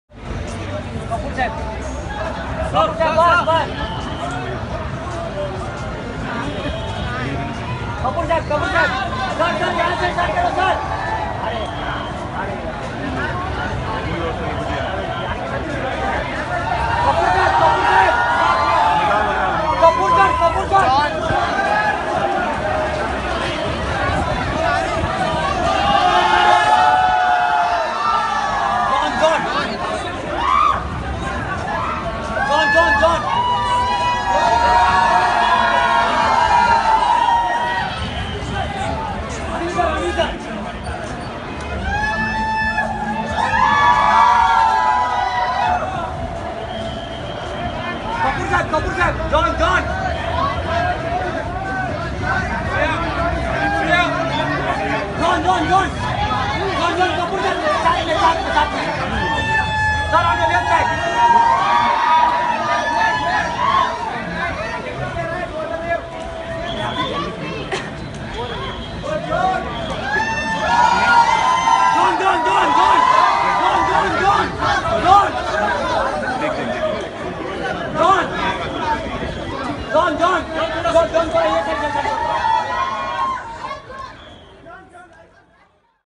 Anil Kapoor and John Abraham at Sachin: A Billion Dreams Premiere | SpotboyE
Bollywood Celebs and Indian cricket team attends Sachin: A Billion Dreams Special Screening.